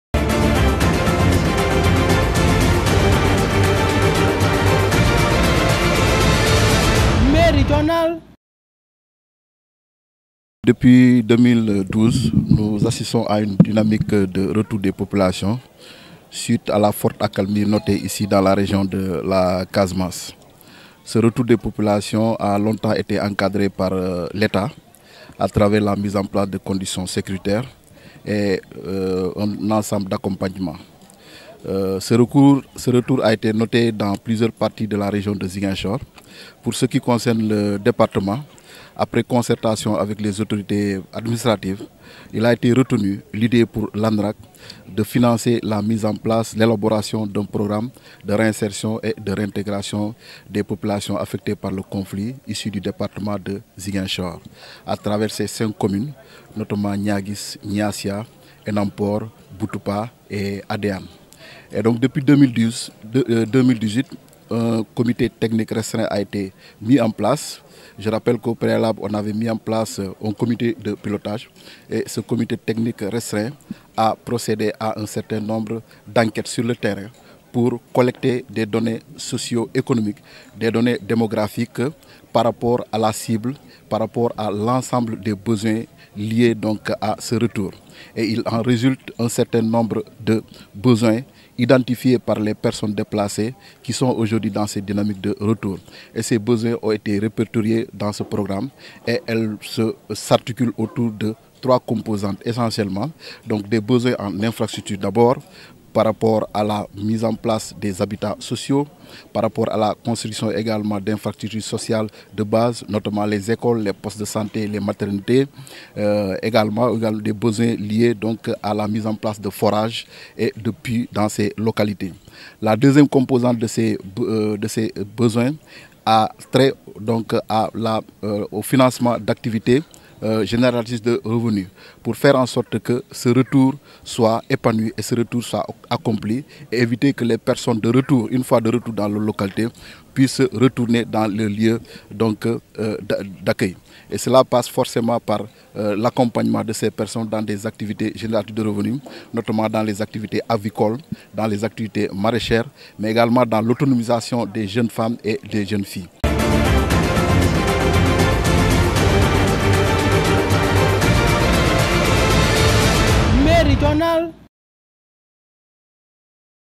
40 MILLIARDS SERONT MOBILISES POUR LA RÉINSERTION DES POPULATIONS AFFECTÉES PAR LE CONFLIT
ZIGUINCHOR:ZIGUINCHOR : AU MOINS 40 MILLIARDS SERONT MOBILISES POUR LA REINSERTION ET LA REINTEGRATION DES POPULATIONS AFFECTEES PAR LE CONFLIT EN CASAMANCE.